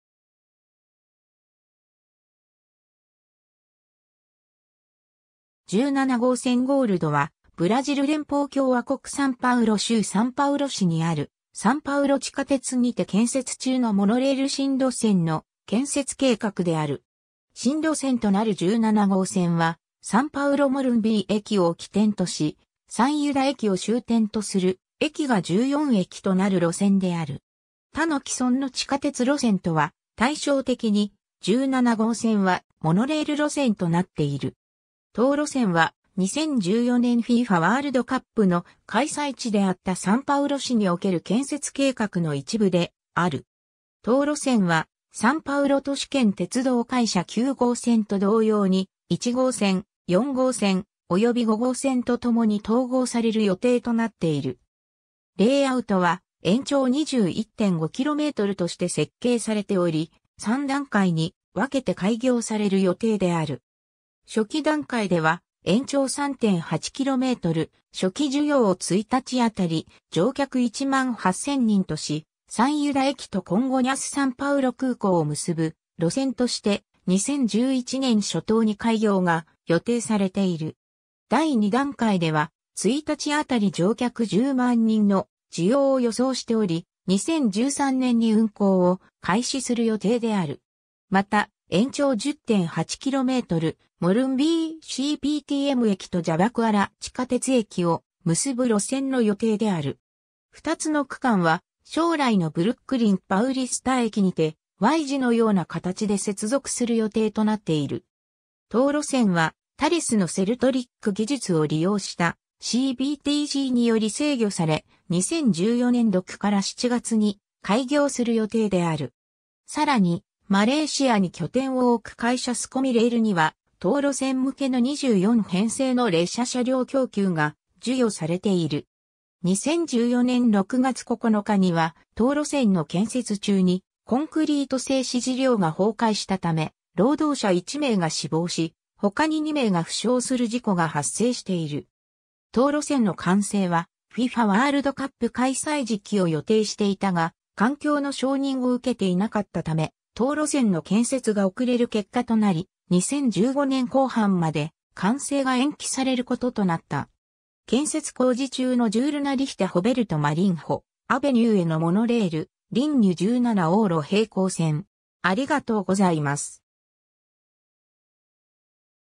サンパウロ地下鉄17号線
17号線 - ゴールド（17ごうせんゴールド、英: Line 17 - Gold、葡: Linha 17 - Ouro）は、ブラジル連邦共和国サンパウロ州サンパウロ市にあるサンパウロ地下鉄にて建設中のモノレール新路線の建設計画である。
 新路線となる17号線は、サンパウロ＝モルンビー駅を起点とし、サンユダ駅を終点とする、駅が14駅となる路線である。
 他の既存の地下鉄路線とは対照的に、17号線はモノレール路線となっている。
 当路線は、2014年FIFAワールドカップの開催地であったサンパウロ市における建設計画の一部である。
 当路線は、サンパウロ都市圏鉄道会社 (CPTM) 9号線（エメラルド）と同様に、1号線（ブルー）、4号線（イエロー）および5号線（ライラック）とともに統合される予定となっている。
 レイアウトは、延長21.5kmとして設計されており、3段階に分けて開業される予定である。
 初期段階では、延長3.8km、初期需要を1日当たり乗客18,000人とし、サンユダ駅とコンゴニャス＝サンパウロ空港を結ぶ路線として、2011年初頭に開業が予定されている。
 第二段階では、1日当たり乗客100,000人の需要を予想しており、2013年に運行を開始する予定である。
また、延長10.8km、モルンビーCPTM駅とジャバクアラ地下鉄駅を結ぶ路線の予定である。
2つの区間は、将来のBrooklin Paulista駅にて「Y」字のような形で接続する予定となっている。
 当路線は、タレスのSelTrac技術を利用したCBTCにより制御され、2014年6-7月に開業する予定である。
 さらに、マレーシアに拠点を置く会社スコミ・レールには、当路線向けの24編成の列車車両供給が授与されている。
 2014年6月9日には、当路線の建設中にコンクリート製支持梁が崩壊したため、労働者1名が死亡し、他に2名が負傷する事故が発生している。
 当路線の完成は、FIFAワールドカップ開催時期を予定していたが、環境の承認を受けていなかったため、当路線の建設が遅れる結果となり、2015年後半まで完成が延期されることとなった。
 建設工事中のJornalista Roberto Marinho Avenueへのモノレール「Linha 17 - Ouro」平行線（2015年2月現在）